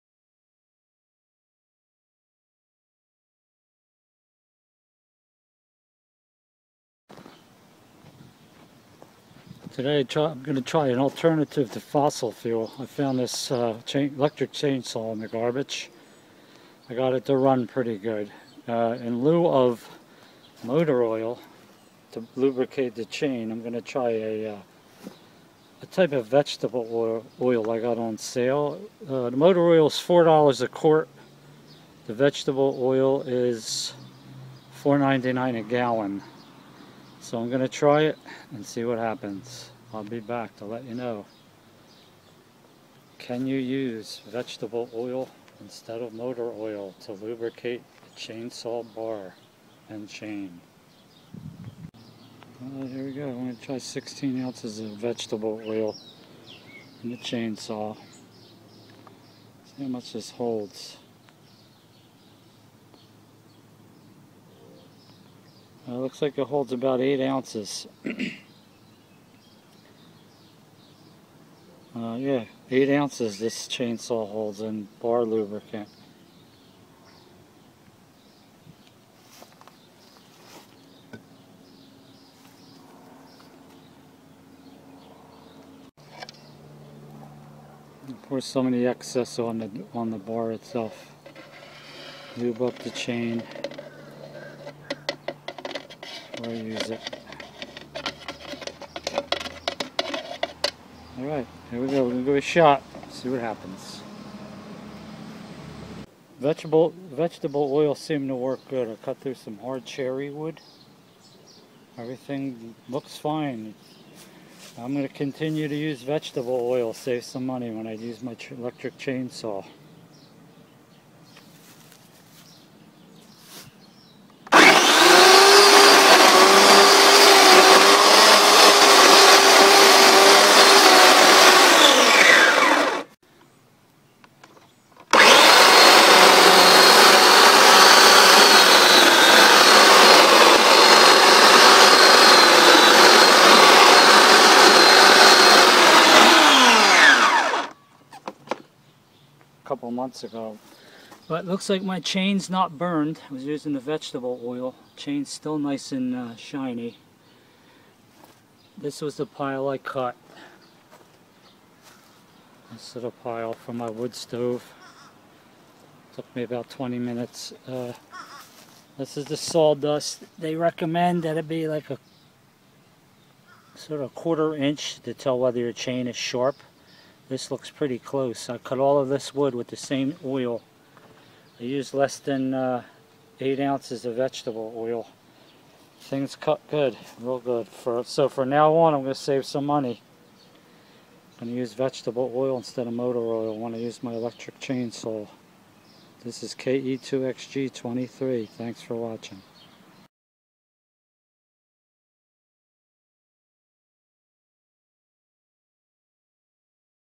Using Vegetable Oil for Chainsaw lubricant
Rather than using Gasoline, in this video I experimented using Vegetable oil in order to lubricate my electric chainsaw chain in the hopes that it would save me some money.